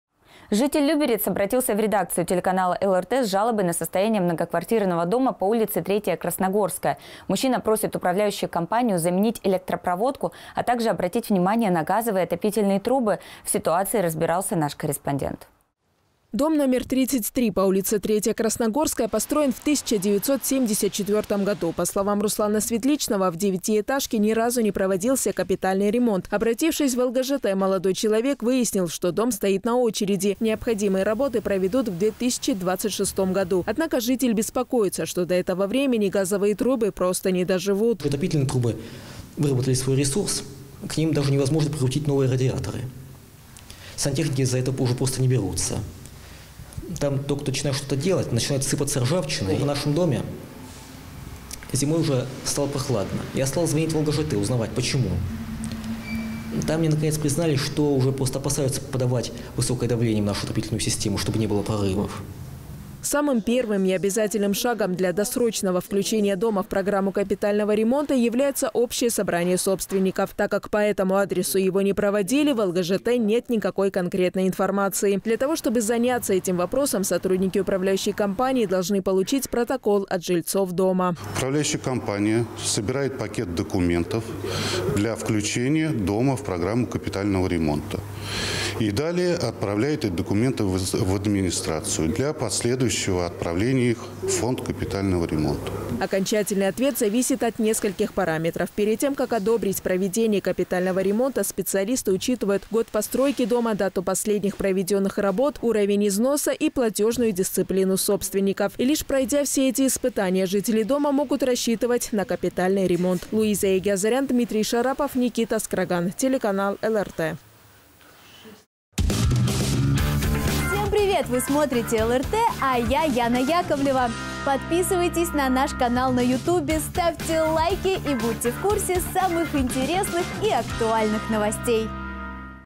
Жители Люберец недовольны состоянием своего дома
Житель Люберец обратился в редакцию телеканала ЛРТ  с жалобой на состояние многоквартирного дома по улице 3-я Красногорская. Мужчина просит управляющую компанию заменить электропроводку, а также обратить внимание на газовые и отопительные трубы. В ситуации разбирался наш корреспондент.

Дом No 33 по улице 3-я Красногорская построен в 1974 году. По словам Руслана Светличного, в 9-этажке ни разу не проводился капитальный ремонт. Обратившись в ОАО "ЛГЖТ", молодой человек выяснил, что дом стоит на очереди. Необходимые работы проведут в 2026 году. Однако житель беспокоится, что до этого времени газовые трубы просто «не доживут».

Руслан Светличный, житель дома No33 по ул. 3-я Красногорская, г. Люберцы:
"Отопительный трубы выработали свой ресурс, к ним невозможно даже подкрутить новые радиаторы, сантехники за это уже просто не берутся. Когда начинают что-то делать, начинает сыпаться ржавчина. Зимой в нашем доме стало прохладно, я позвонил в ЛГЖТ. Мне признались, что просто опасаются подавать высокое давление в нашу отопительную систему, чтобы не было прорывов".

Самым первым и обязательным шагом для досрочного включения дома в программу капитального ремонта является общее собрание собственников. Так как по этому адресу его не проводили, в ОАО "ЛГЖТ" нет никакой конкретной информации. Для того, чтобы заняться этим вопросом, сотрудники управляющей компании должны получить протокол от жильцов многоквартирного дома.

Сергей Чикин, заместитель генерального директора ОАО «ЛГЖТ»:
"Управляющая компания собирает пакет документов для включения дома в программу капитального ремонта, и отправляет эти документы в администрацию для последующего отправления их в фонд капитального ремонта".

Окончательный ответ зависит от нескольких параметров. Перед тем, как одобрить проведение капитального ремонта, специалисты учитывают год постройки дома, дату последних проведенных работ, уровень износа и платежную дисциплину собственников. И лишь пройдя все эти испытания, жители дома могут рассчитывать на капитальный ремонт.

Луиза Егиазарян. Дмитрий Шарапов. Никита Скраган